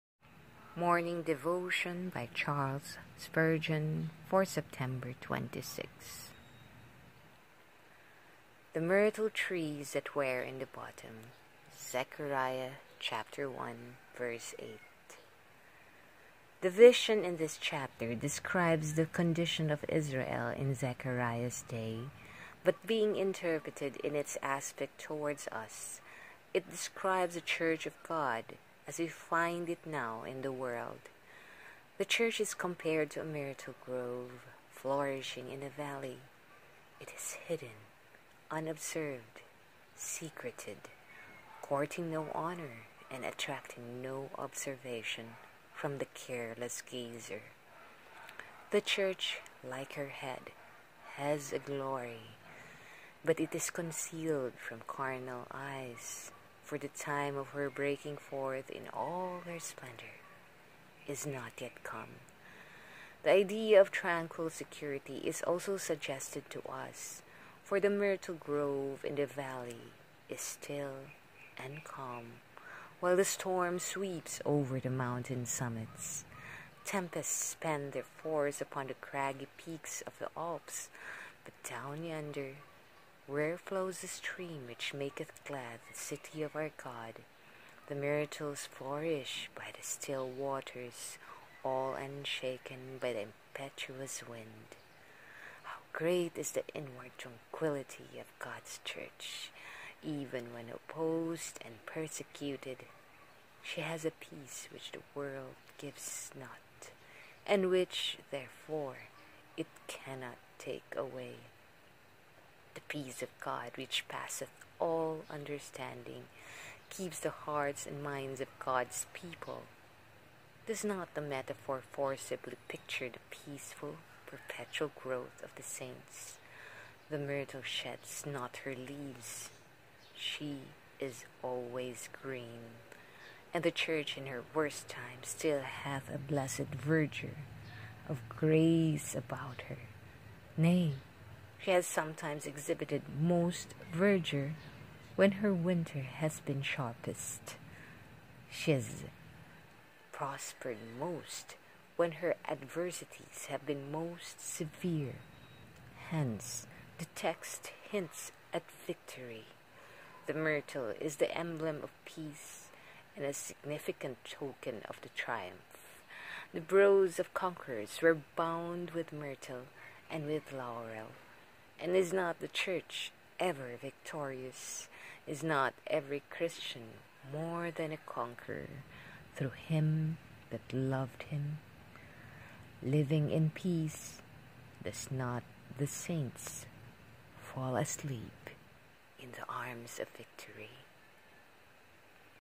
Morning Devotion for September 26 by Charles Spurgeon
Source: Morning and Evening Devotion by Charles Spurgeon
Read by: Bing Veloso

“The myrtle trees that were in the bottom.”
 - Zechariah 1:8

The vision in this chapter describes the condition of Israel in Zechariah’s day;

but being interpreted in its aspect towards us,

it describes the Church of God
as we find it now in the world.

The Church is compared to a myrtle grove
flourishing in a valley.
It is hidden,
unobserved,
secreted;
courting no honour
and attracting no observation from the careless gazer.

The Church,
like her head, has a glory,
but it is concealed from carnal eyes,

for the time of her breaking forth
in all her splendour
is not yet come.

The idea of tranquil security
is also suggested to us:

for the myrtle grove in the valley

is still

and calm,

while the storm sweeps over the mountain summits.

Tempests spend their force upon the craggy peaks of the Alps,
but down yonder where flows the stream which maketh glad the city of our God,

the myrtles flourish by the still waters,

all unshaken by the impetuous wind.

How great is the inward tranquility of God’s Church!
Even when opposed and persecuted,
she has a peace which the world gives not,

and which,
therefore,
it cannot take away:

the peace of God which passeth all understanding
keeps the hearts and minds of God’s people.

Does not the metaphor forcibly picture
the peaceful, perpetual growth of the saints?

The myrtle sheds not her leaves,
she is always green;
and the Church
in her worst time
still hath a blessed verdure of grace about her;

nay,
she has sometimes exhibited most verdure
when her winter has been sharpest.

She has prospered most
when her adversities have been most severe.

Hence
the text hints at victory.

The myrtle
is the emblem of peace,
and a significant token of triumph.

The brows of conquerors
were bound with myrtle and with laurel;
and is not the Church
ever victorious?
Is not every Christian
more than a conqueror
through Him that loved him?

Living in peace,
do not the saints fall asleep
in the arms of victory?

- Charles Spurgeon